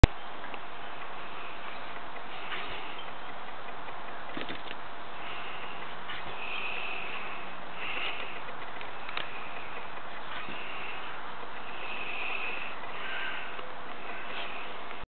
Old English Sheepdog Puppies
Four weeks old. Old English Sheepdog Puppies in the yard with Buddy the maltese.